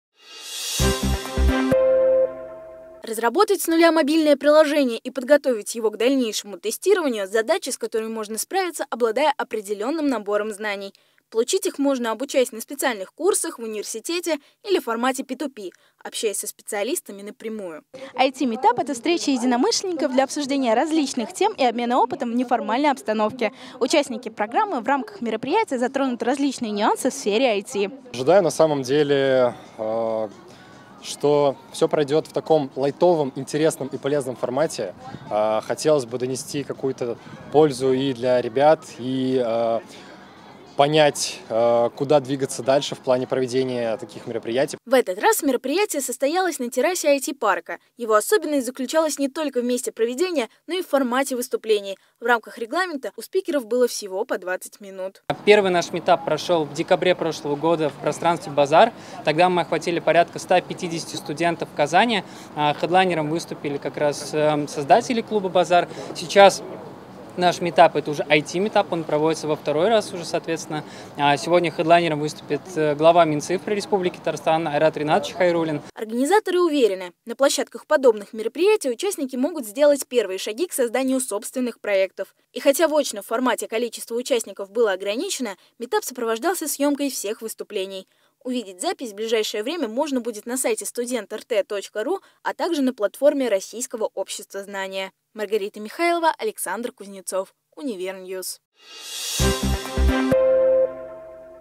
IT-MEET UP ПОД ОТКРЫТЫМ НЕБОМ
На минувшей неделе на площадке казанского IT-парка состоялся открытый Meet up для студентов. Все подробности выясняла Маргарита Михайлова.